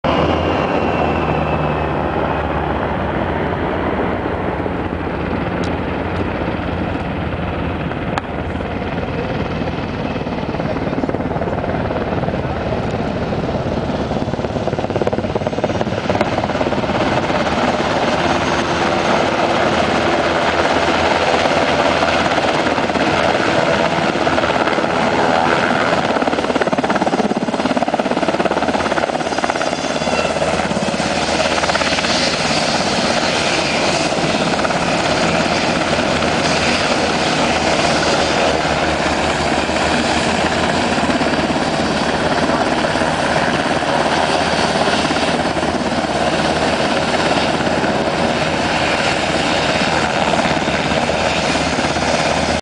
Heli fires off flares
Helicopter fires off flares as it comes in to land.